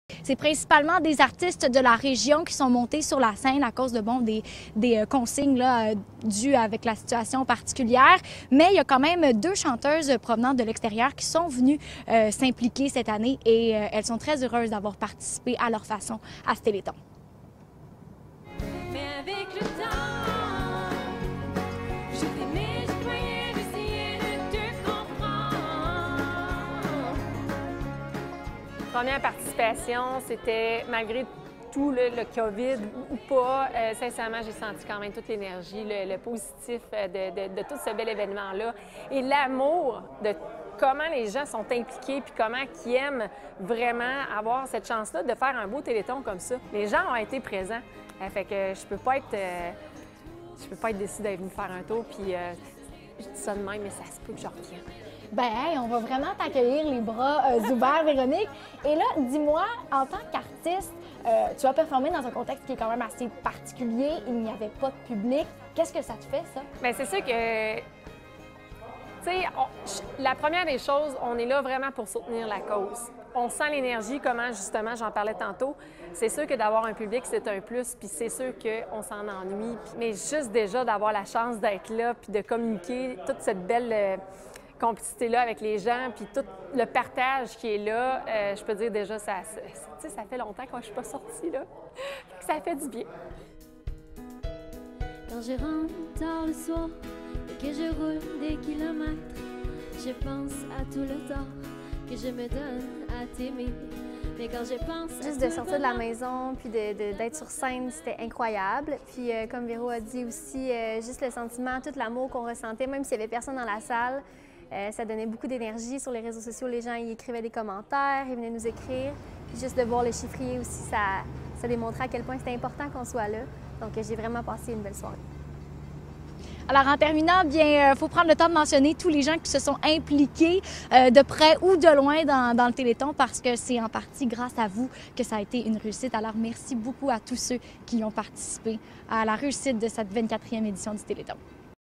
Topo TVA Téléthon la Ressource Véronique Labbé & Laurence St-Martin
Topo et courte entrevue de TVA à propos du téléthon la Ressource de Rouyn-Noranda.